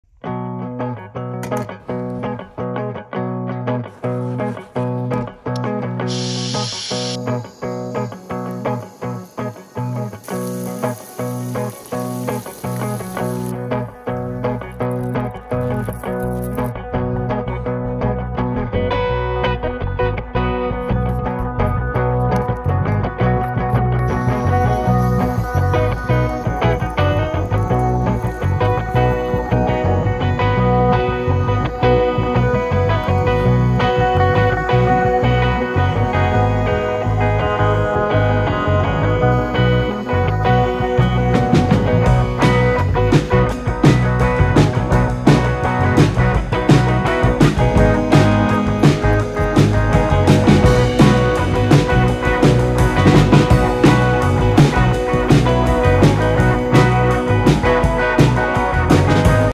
Spaghetti Carbonara
Escape to the great outdoors with our Spaghetti Carbonara recipe video set in a charming picnic area. Immerse yourself in the beauty of nature as we guide you through the art of creating the perfect Carbonara.